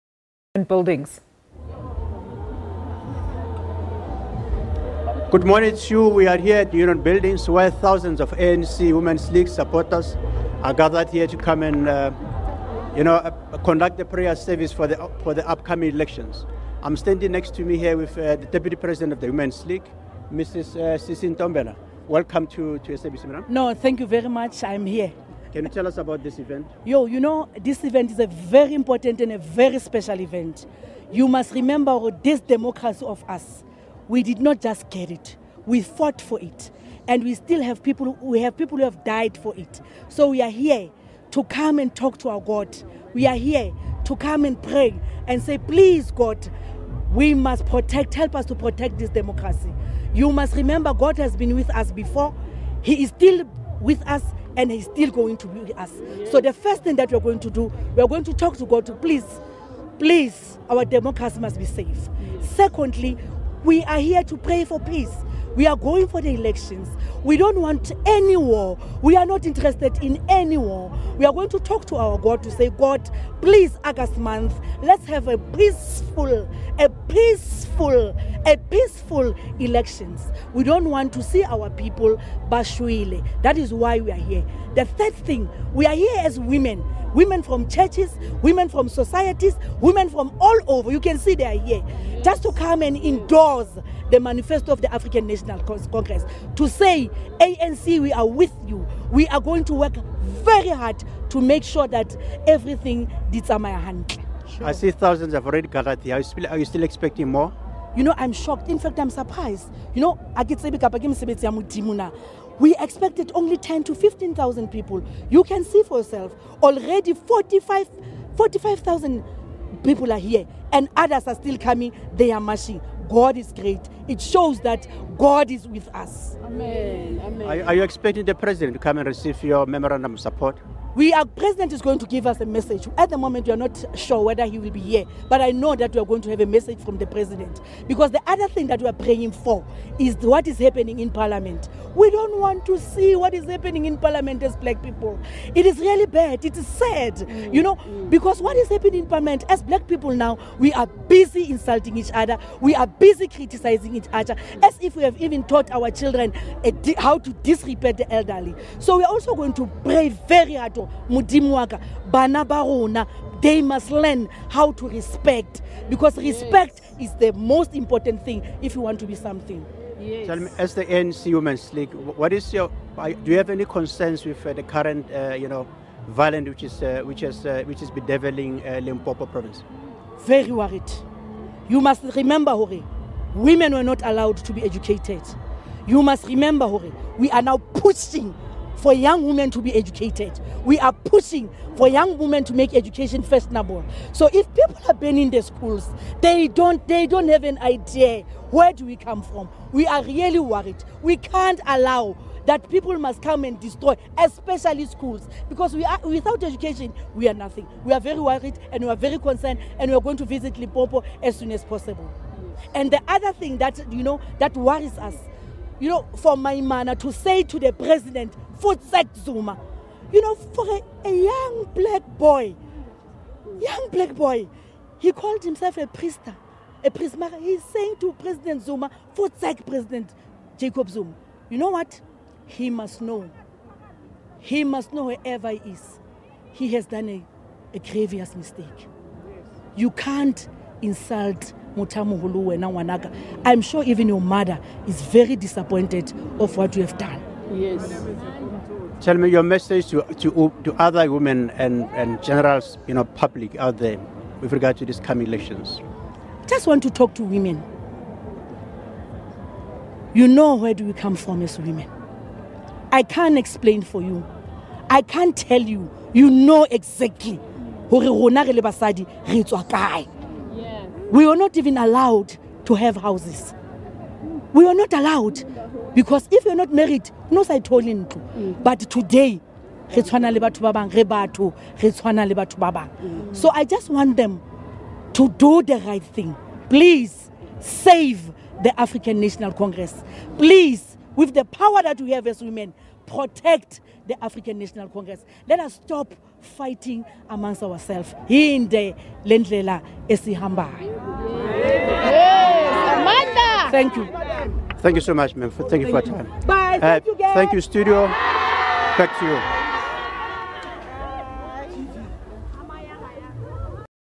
ANCWL Dep. President Sisi Ntombela outlines purpose of the march
ANC Women's League is expected to hold a peaceful march to Union Buildings in Pretoria on Saturday. For more details on the March, we now cross to our reporter, MONTLENYANE DIPHOKO, who is at the Union Buildings.